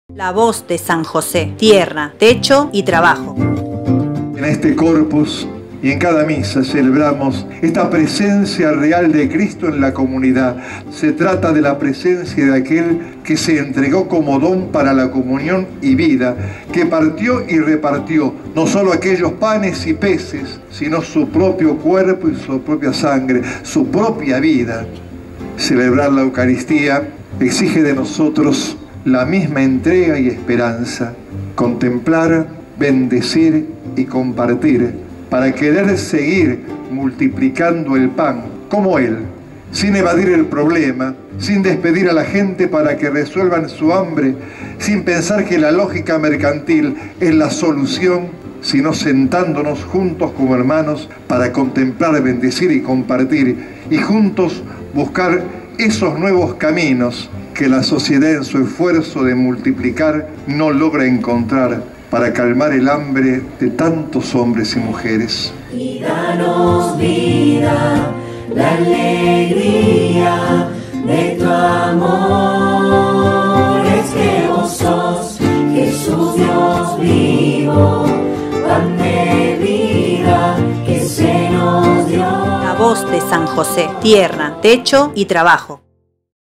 Corpus Christi 2022
Las comunidades de la diócesis de San Justo realizaron la peregrinación de Corpus Christi hasta la Catedral en la que se llevo a cabo la misa y la tradicional procesión por las calles de la ciudad.
Además, se compartió un momento emotivo con la presentación de un video homenaje al Padre Bachi, a la Hermana Clementina y al Padre Gregorio Zitzman que ya han partido a la presencia del Padre.
Luego, se cerró la jornada con un festival de música popular, con la presencia de artistas como Las Voces del Alma, Manantial de Luz y Facundo Marlia.